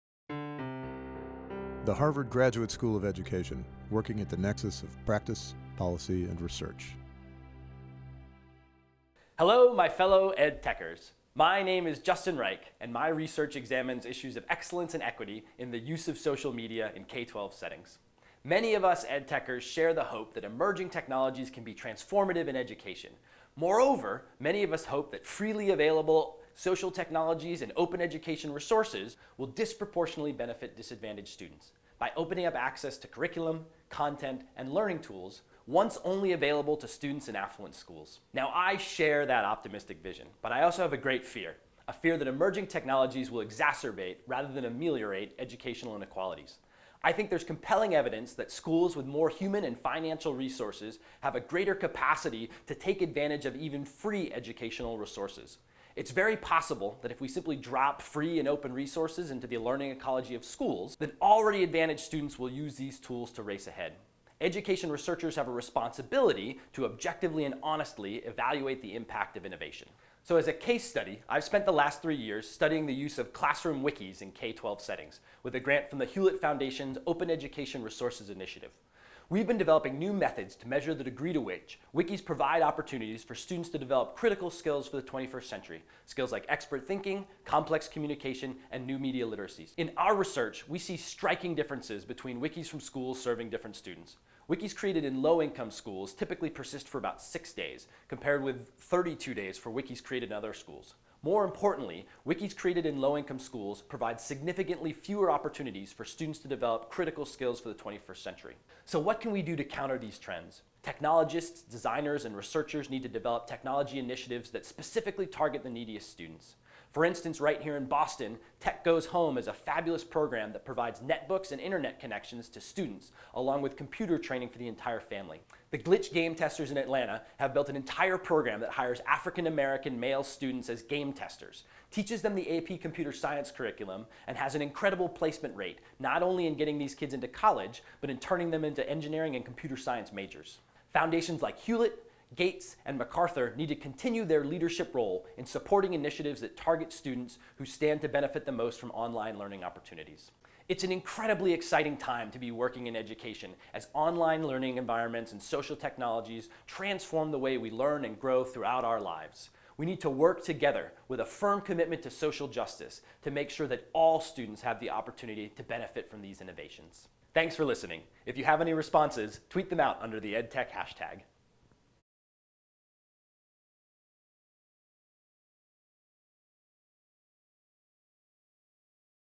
Dear edtech: Thoughts on Digital Equity
Ed.D. candidate Justin Reich discusses his HGSE research with the edtech community.